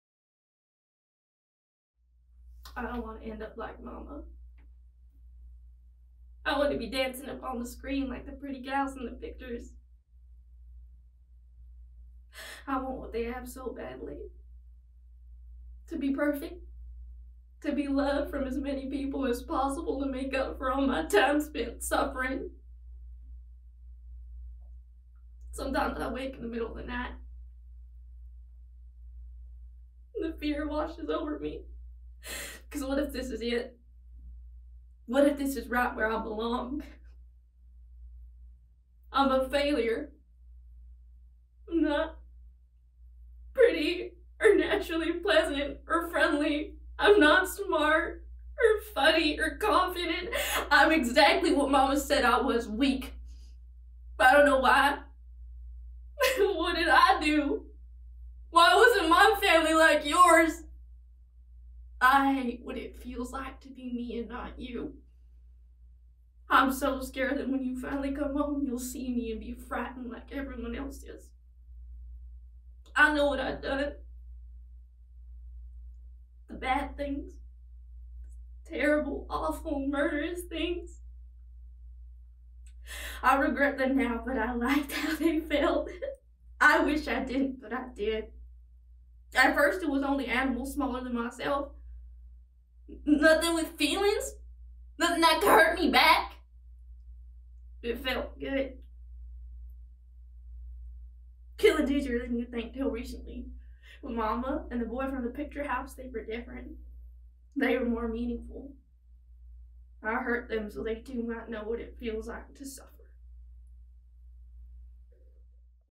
SASCARS 2024 | Best Dramatic Monologue Nominee | Gisela Guajardo | Pearl
Gisela Guajardo's Nomination for Best Dramatic Monologue

Actress: Gisela Guajardo
Film: Pearl (2022), dir. Ti West
Role: Pearl

The 4th annual SASCARS will be held on Saturday, April 13th!